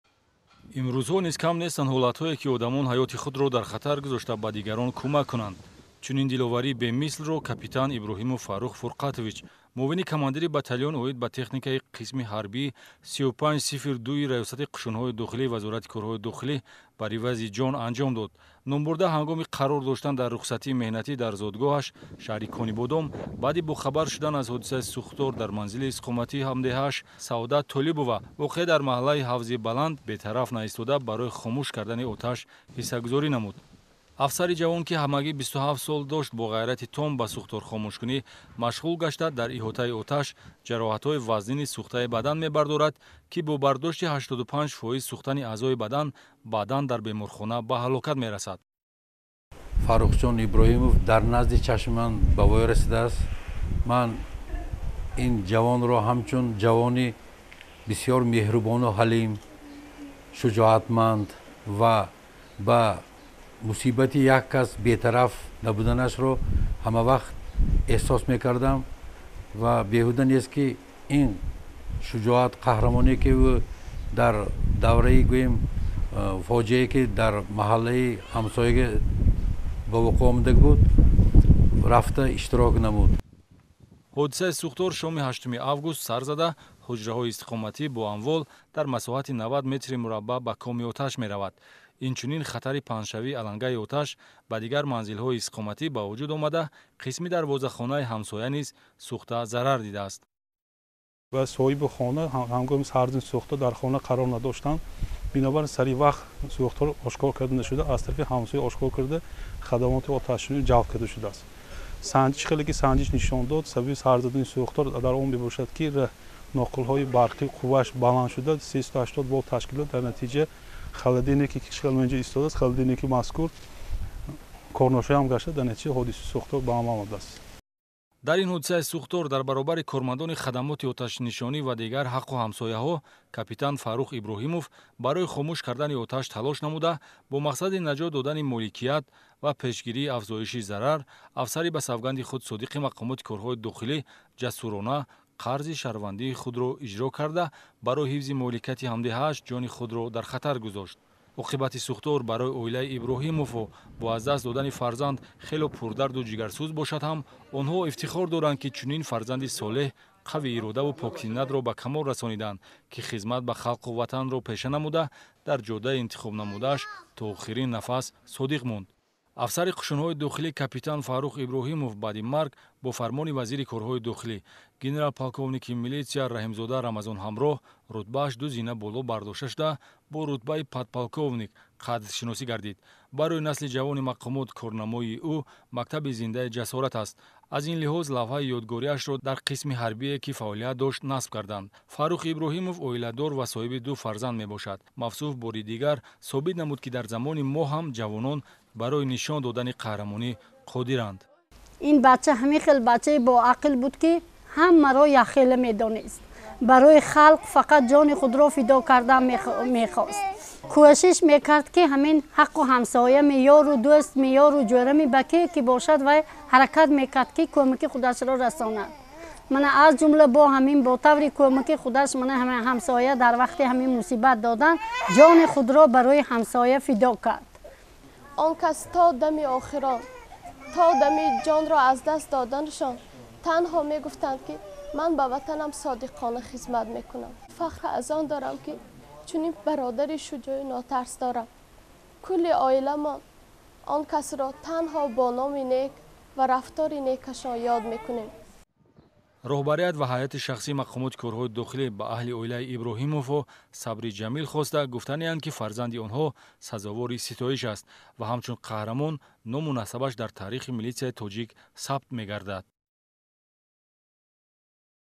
Фоҷеа дар Конибодом ва қаҳрамонии капитан Иброҳимов
Капитани милитсия Фаррух Иброҳимов 8 август дар шаҳри Конибодом кӯдаки ҳамсояро аз дами оташ наҷот дода, дар вақти фурӯ рафтани боми хона худ ба коми оташ афтод. Табибон ташхис кардаанд, ки 85 фоизи бадани вай сӯхтааст.